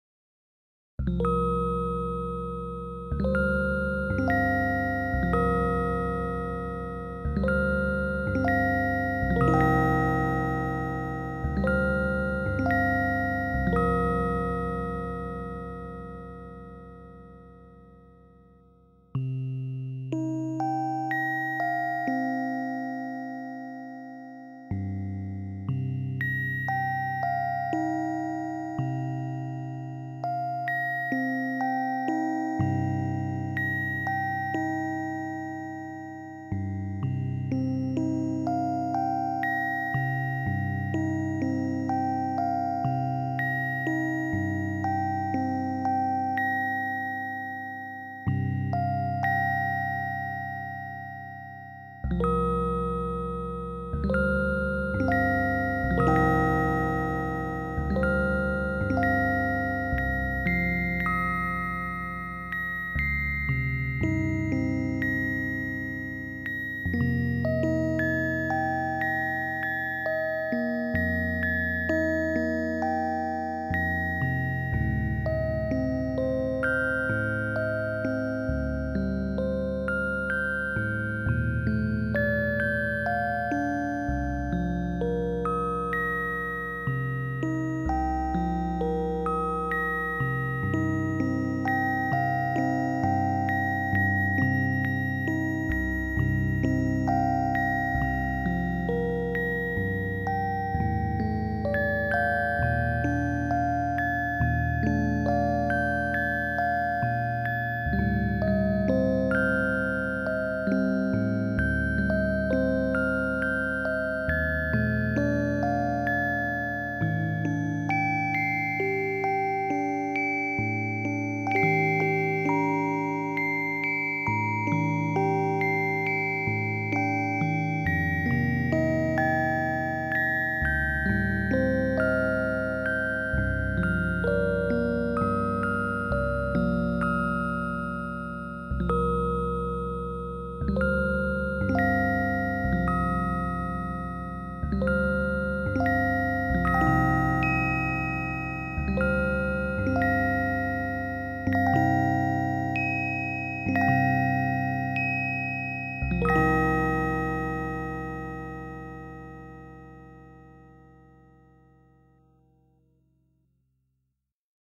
Laptop Improvisation in 7ED2:1
A short improvisation using my laptop's keyboard while running a Max/MSP patch "n-ET Microtonal Keyboard" I programmed myself. In this improvisation I'm dividing the perfect octave (ratio 2:1) in 7 equal parts.

Laptop: Pedro F. Finisterra

Illustration: Pedro F. Finisterra

3rd of February, 2019
Fazendas de Almeirim, Portugal